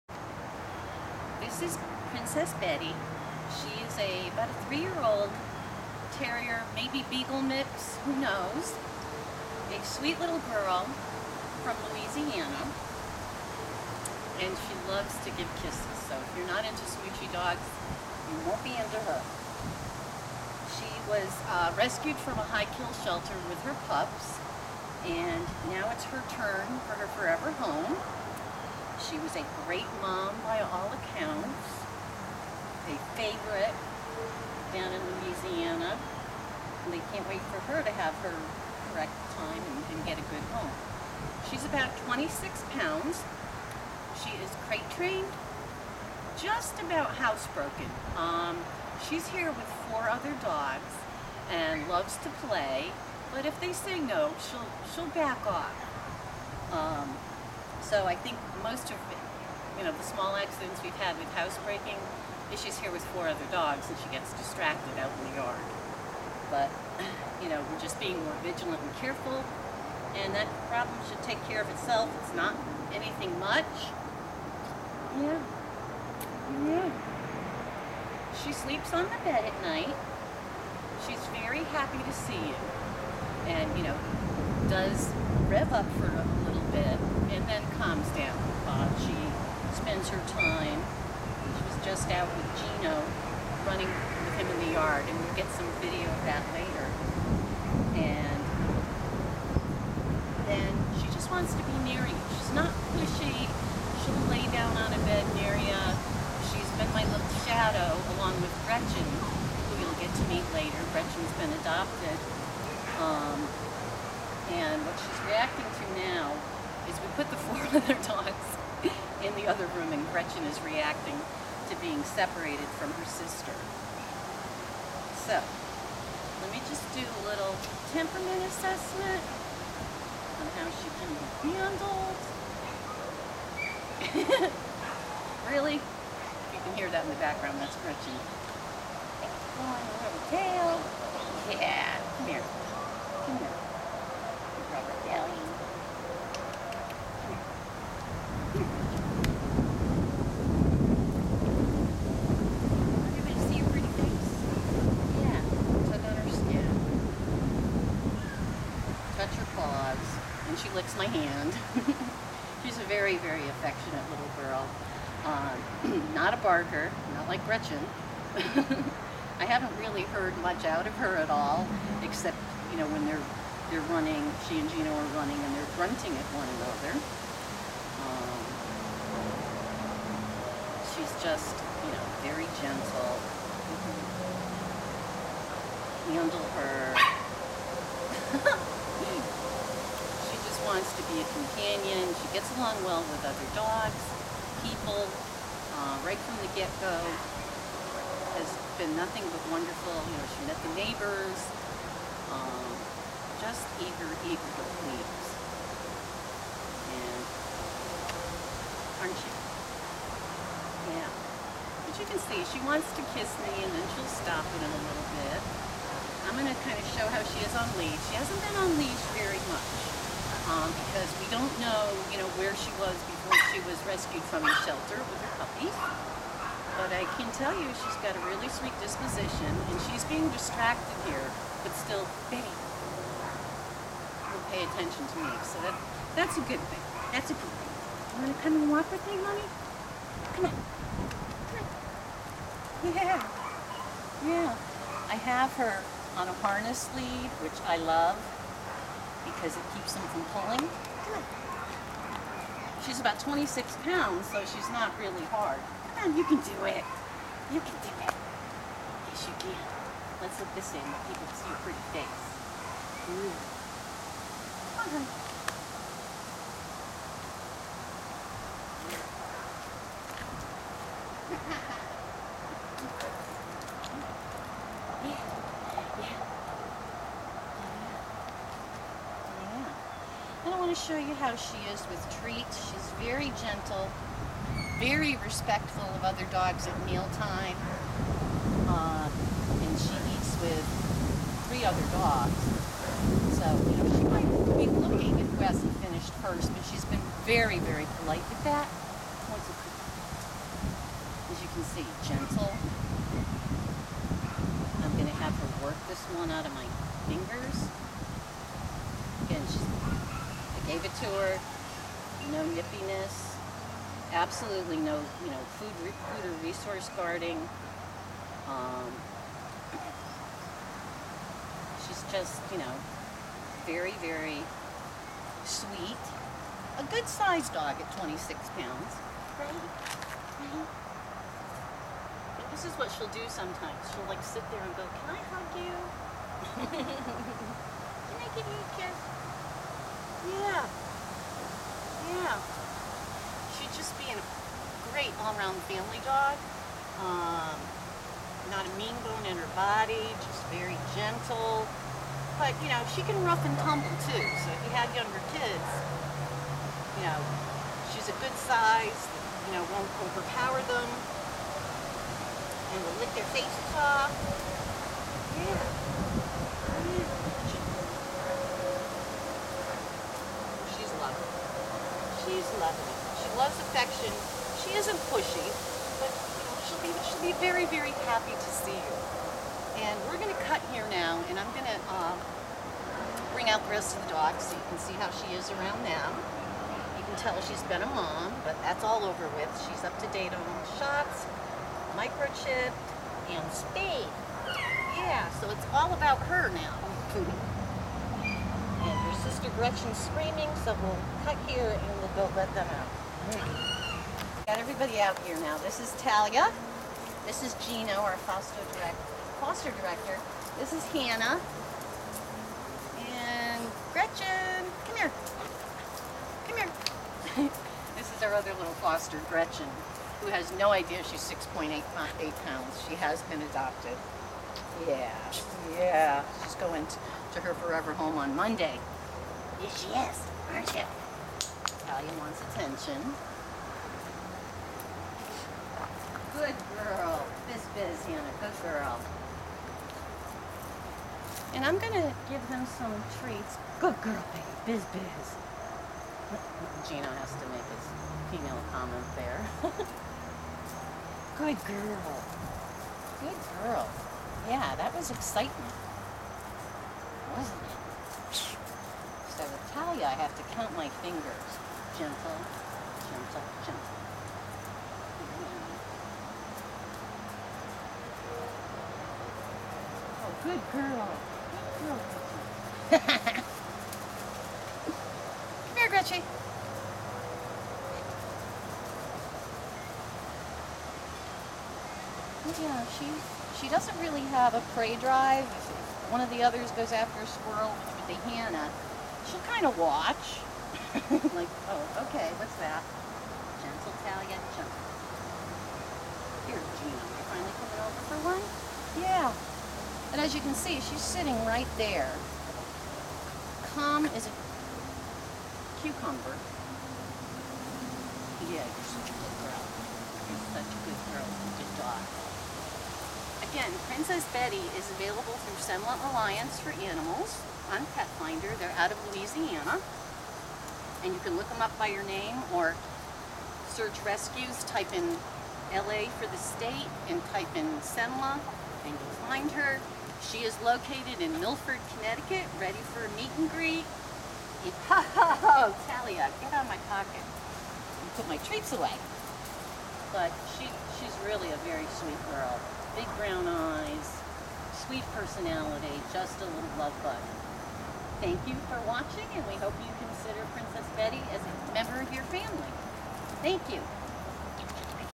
Princess Betty's adoption video
Princess Betty is available for adoption through Cenla Alliance for Animals.  She is being fostered in Milford, CT.